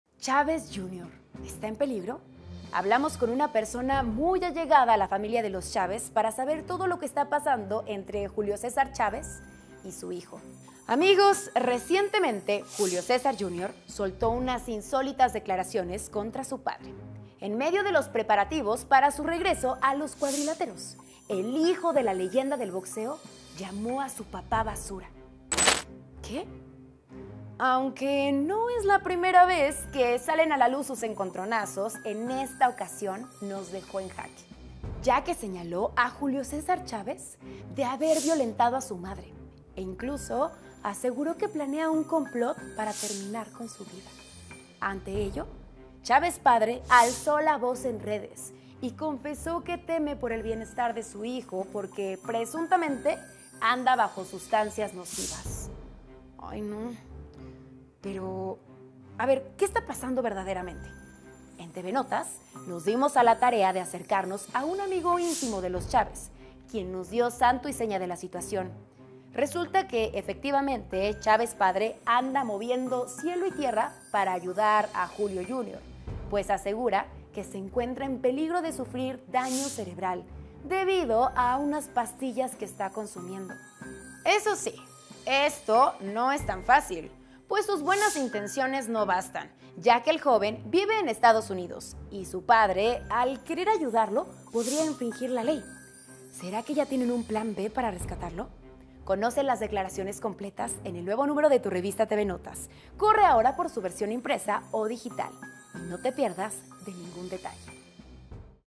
¿Chávez Jr. está en peligro?
Julio César Chávez lucha contra las leyes de EU para ayudar a su hijo: “Mientras yo viva, no me voy a cansar”. 😰